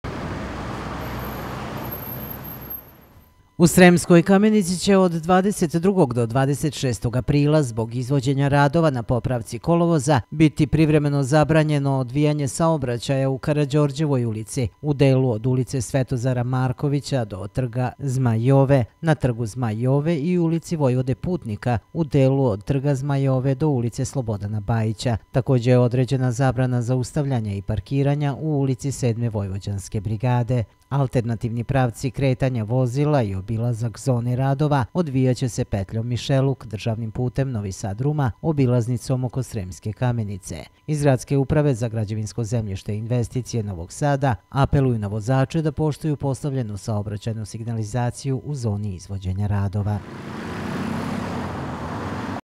TV KANAL 9, NOVI SAD: RADOVI NA KOLOVOZU U CENTRU SREMSKE KAMENICE
Od ponedeljka počinju radovi na kolovozu u centru Sremske Kamenice zbog kojih će do petka biti zabranjeni saobraćaj i parkiranje u nekoliko ulica.